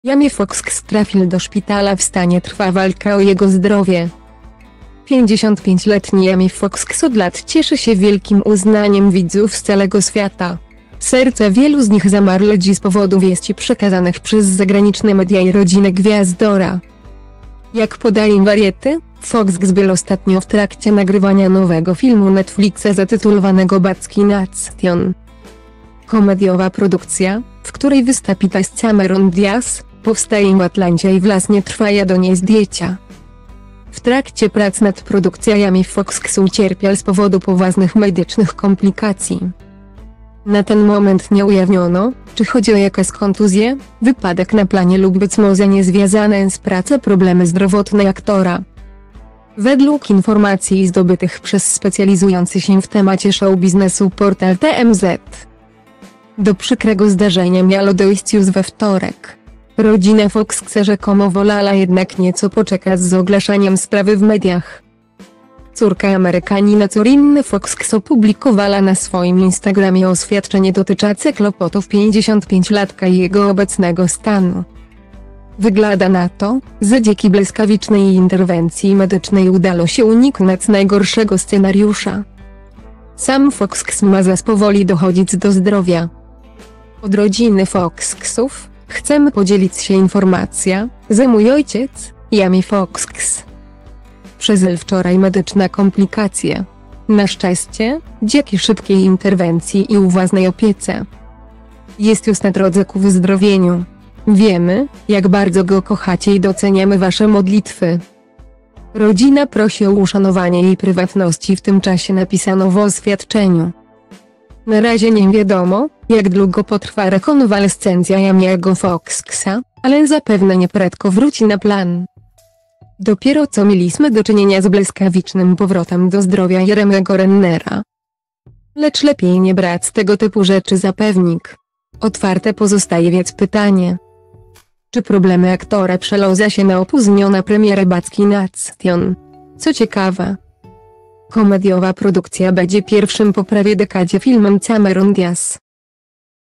Jamie Foxx trafil do szpitala w stanie.Trwa walka o jego zdrowie
55- letni Jamie Foxx od lat cieszy sie wielkim uznaniem widzów z calego swiata. Serca wielu z nich zamarly dzis z powodu wiesci przekazanych przez zagraniczne media i rodzine gwiazdora.Jak podaje Variety, Foxx byl ostatnio w trakcie nagrywania nowego filmu Netflixa zatytulowanego "Back in Action".Komediowa produkcja, w której wystapi tez Cameron Diaz, powstaje w Atlancie i wlasnie trwaja do niej zdjecia.